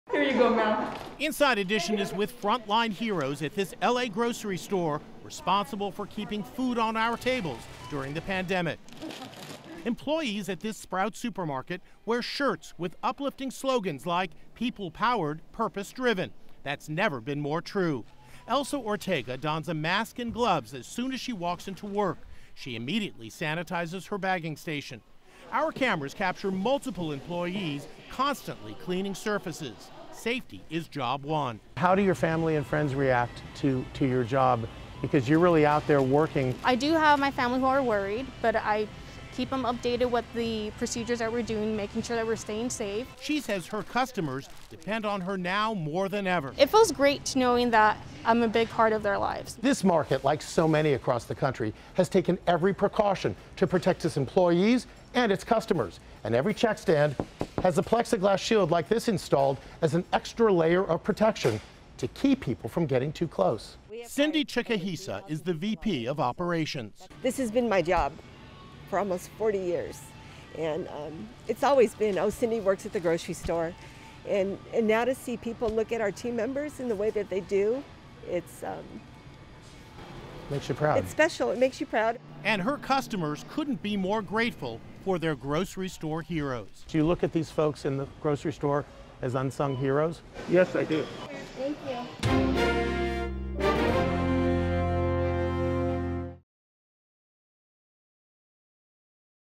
Meet Grocery Store Workers ‘Powered by Purpose’
Inside Edition is with frontline heroes at this Los Angeles area grocery store that thousands of people depend on during the pandemic. Employees at this Sprouts Supermarket wear shirts with uplifting phrases like”People Powered, Purpose Driven.” These dedicated workers put themselves at risk to keep food on their own table while helping their neighbors put food on theirs. We saw how diligently these employees sanitize surfaces and exercise precautions to keep everyone safe while shopping.